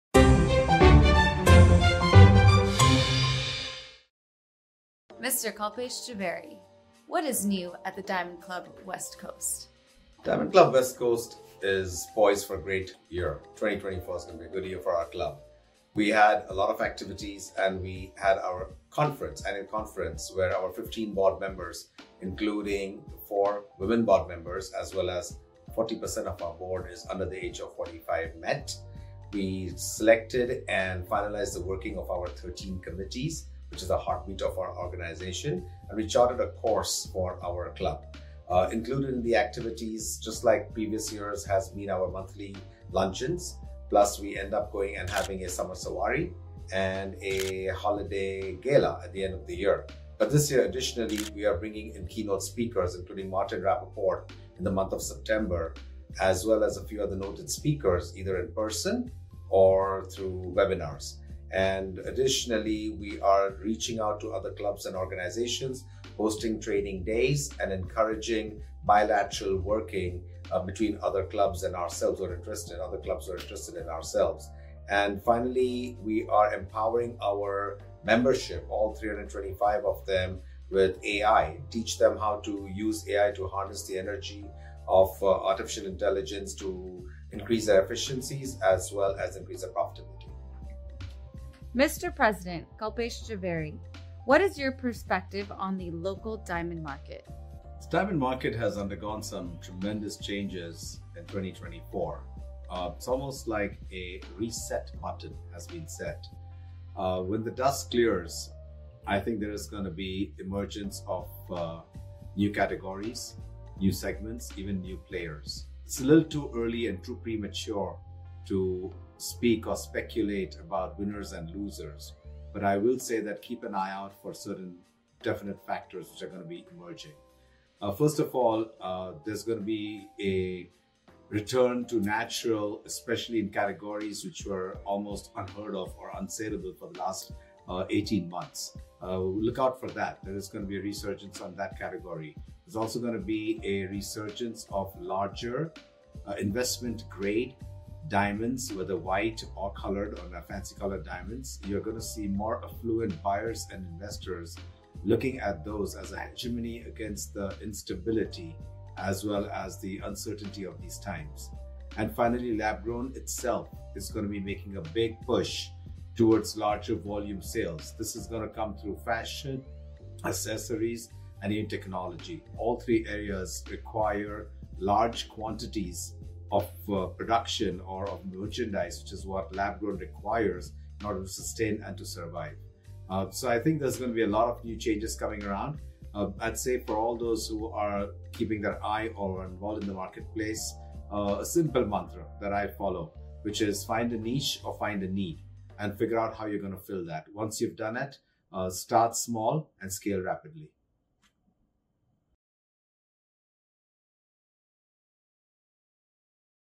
WFDB SERIES : What's Happening @ Diamond Club West Coast - Ft President Kalpesh Jhaveri
Introducing our new series: What's Happening! Join us for a quick, insightful chat with industry leaders. In our first episode, we have Kalpesh Jhaveri, President of Diamond Club West Coast, sharing his thoughts on the current trends in the diamond market. Don't miss out!